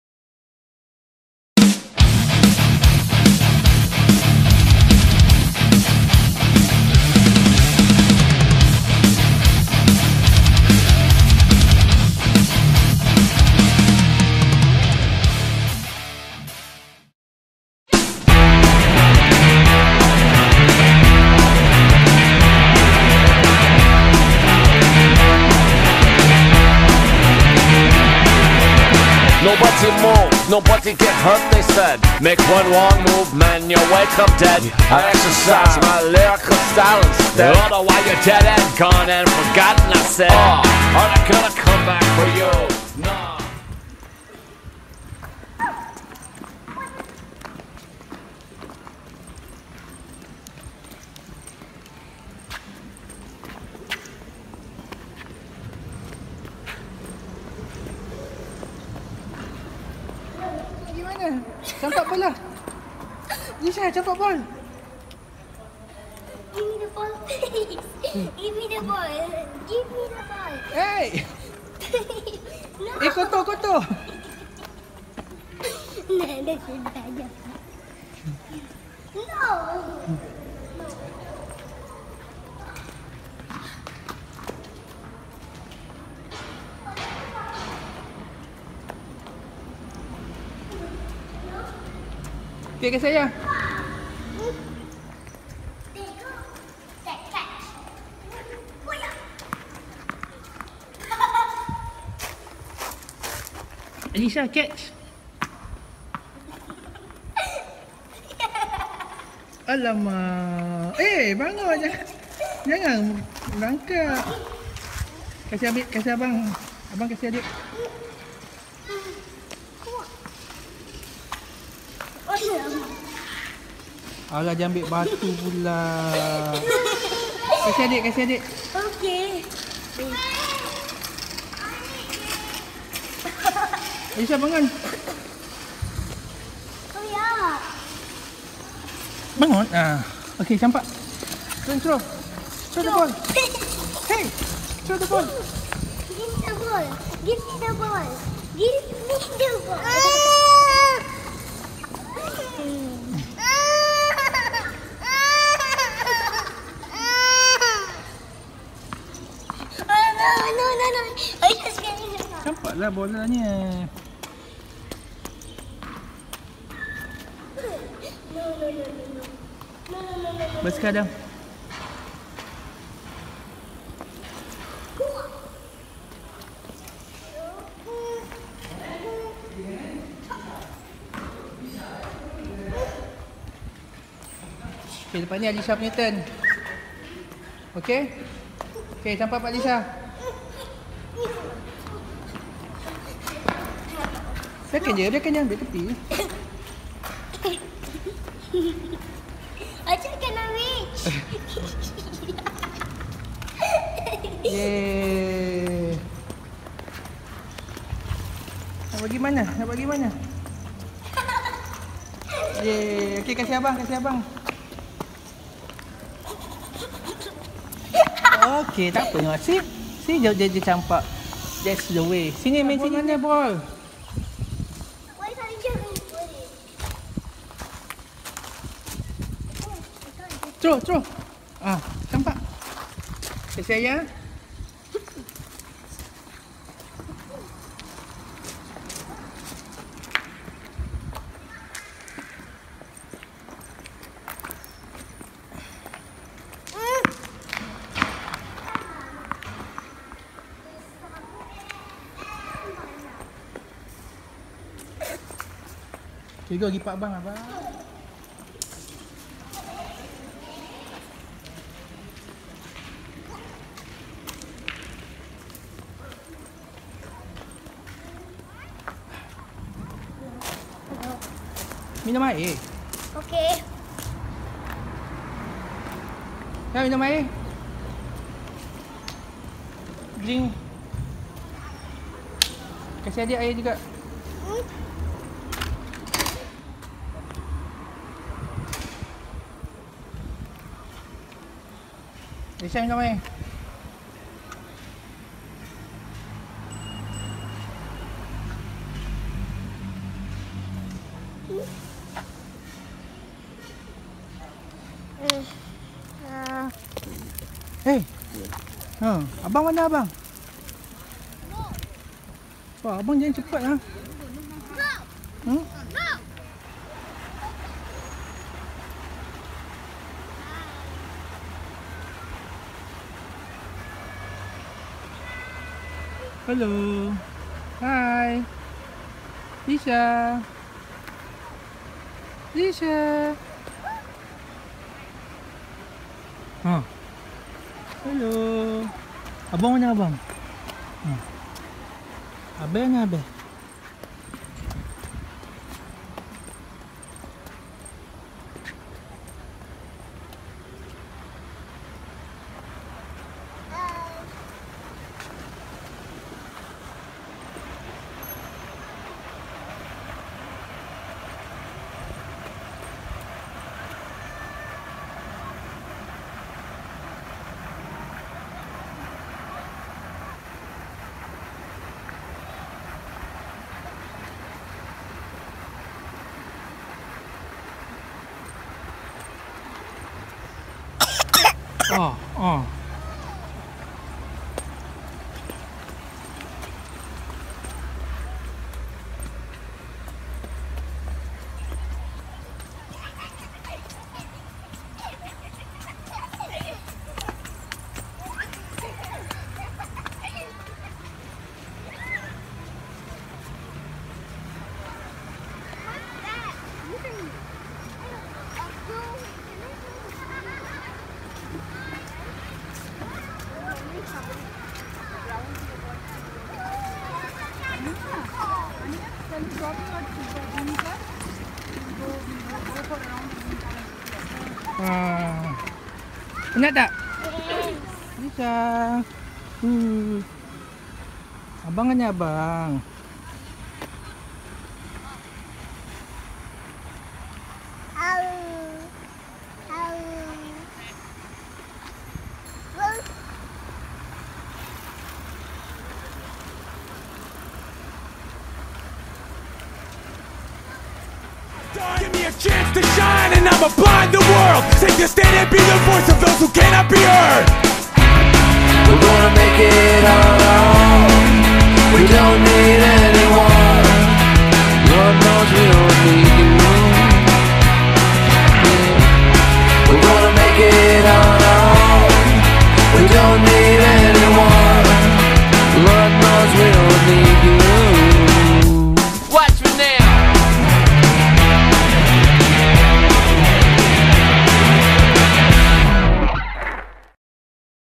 Adam & Nur Alisha @ Punggol Waterway Park - E01
Adam and Alisha were at the Punggol Waterway Park to play with the tennis ball. After that, we went to the promanade to rest while Adam played with his skate scooter.